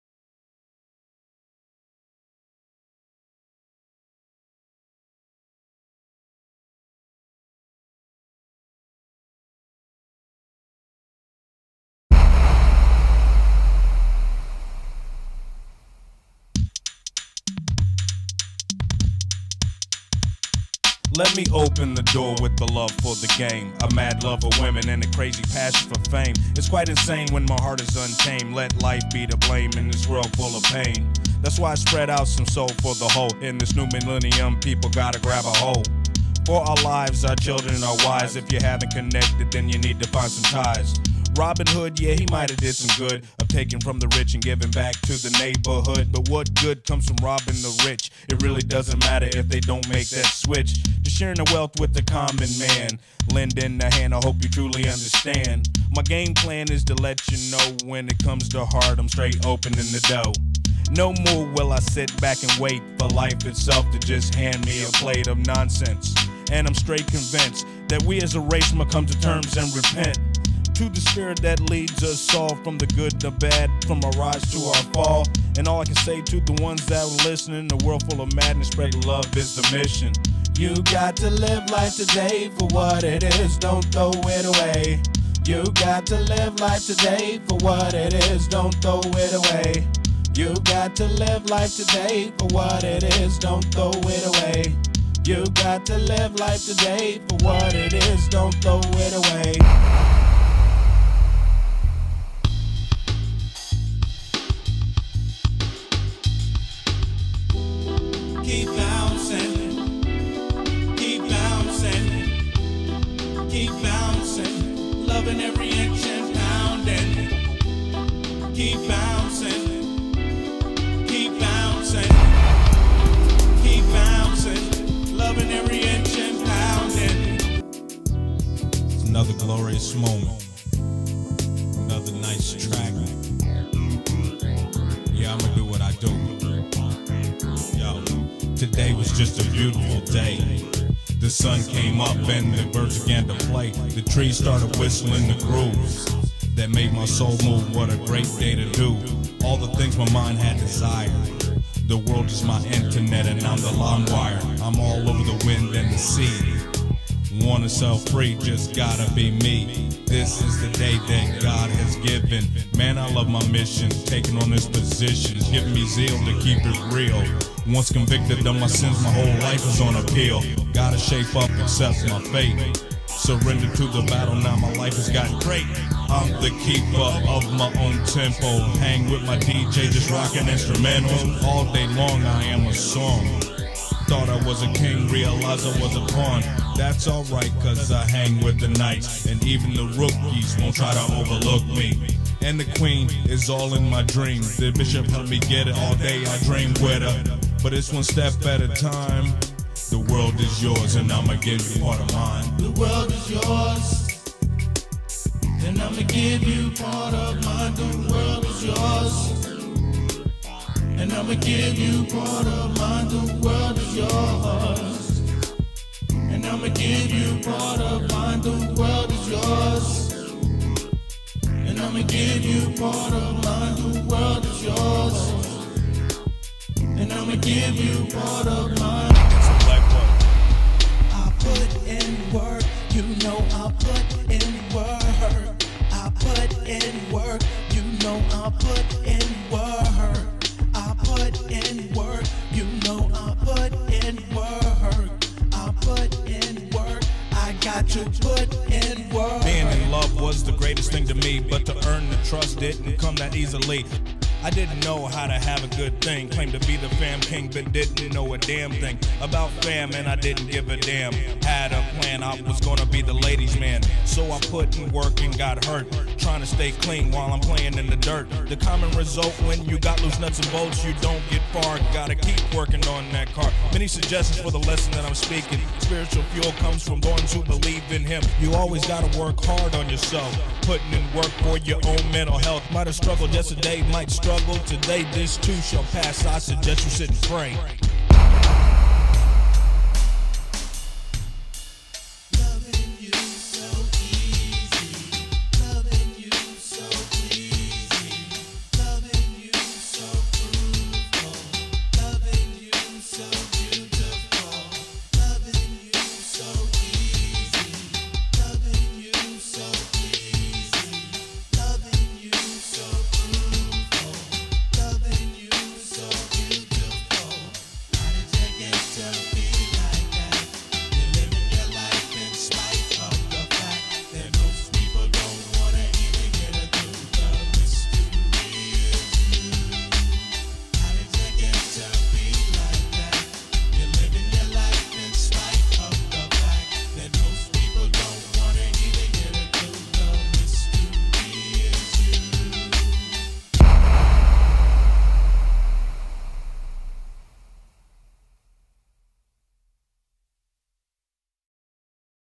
Ell Estill Mini Mix Part 1
Some of Ell Estill's greatest hits are in this mini mix. Happy Birthday Ell, enjoy your day.